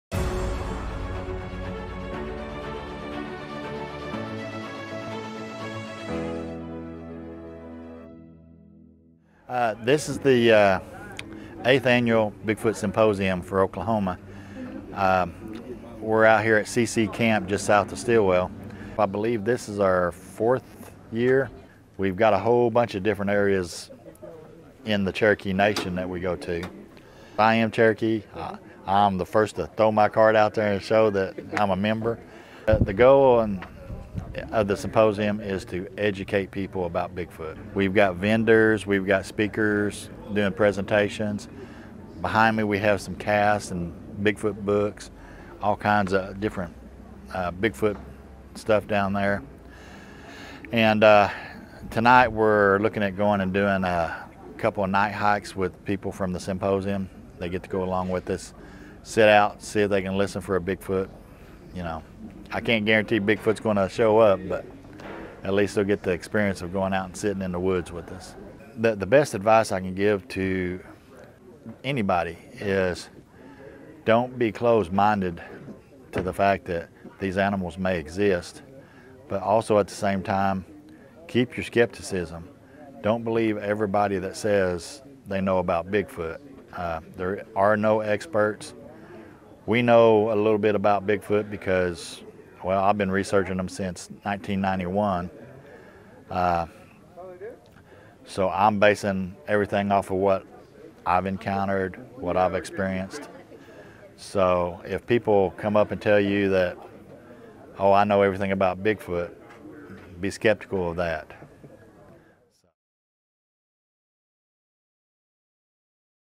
8th Annual Bigfoot Symposium for Oklahoma hosted in Adair County
The 8th Annual Bigfoot Symposium for Oklahoma was hosted at C.C. Camp in Adair County on March 7. D. W. Lee, executive director of the Mid America Bigfoot Research Center, said the symposium’s goal is to educate people on bigfoot. The event included speakers, vendors and a night hike for some symposium attendees.